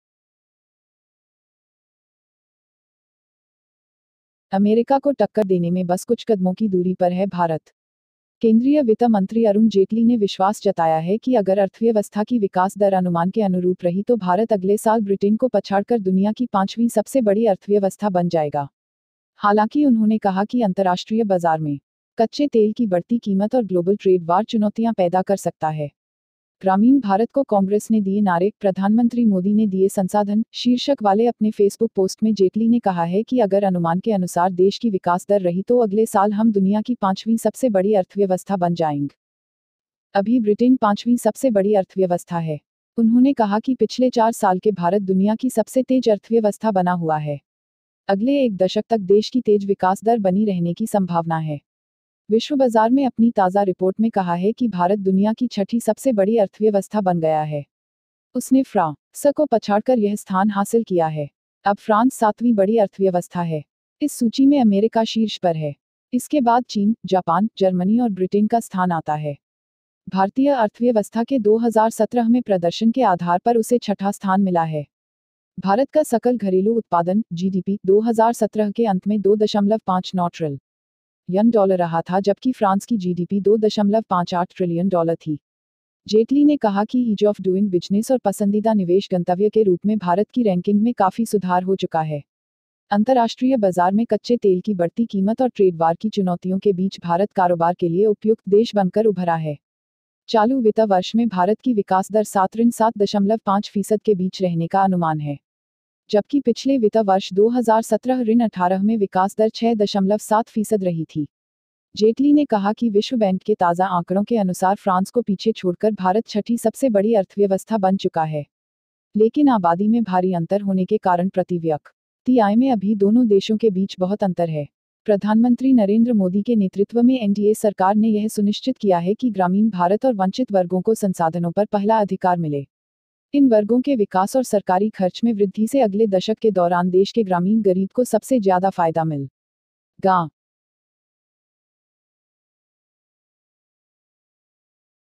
America को टक्कर देने में बस कुछ कदमों की दूरी पर है India
Entry News

Union Finance Minister Arun Jaitley has expressed confidence that if the economy's growth rate is in line with the estimates, India will become the fifth largest economy of the world by defeating Britain next year. However, he said that the rising global crude prices and global trade-war can create challenges in the international market.

In his Facebook post titled "The slogan given by the Congress to the rural India, Prime Minister Modi has given the resource", Jaitley said that if the country's growth rate is estimated, then next year we will become the fifth largest economy in the world. Britain is the fifth largest economy right now. He said that the last four years India has become the world's fastest economy. There is a possibility of continued growth of the country for the next decade.

In its latest report in the global market, it has said that India has become the sixth largest economy in the world. He has achieved this position by overtaking France. Now France is the seventh largest economy. America is on top in this list. This is followed by China, Japan, Germany and Britain. On the basis of performance of Indian economy in 2017, it got sixth place. India's gross domestic product (GDP) was $ 2.59 trillion at the end of 2017, while French GDP was 2.58 trillion dollars.

Jaitley said that India's rankings have improved considerably as the Isle of Doing Business and preferred investment destination. In the current financial year, India's growth rate is estimated to be between 7-7.5 per cent. While the growth rate was 6.7% in the last financial year 2017-18.

Jaitley said that according to the World Bank's latest figures, India has become the sixth largest economy after leaving France. But there is a great difference between the two countries in per capita income due to the huge difference in the population. Under the leadership of Prime Minister Narendra Modi, the NDA government has ensured that rural India and the disadvantaged sections get the first right on resources. With the growth of these sections and the increase in government spending, the rural poor will get the most benefit during the next decade.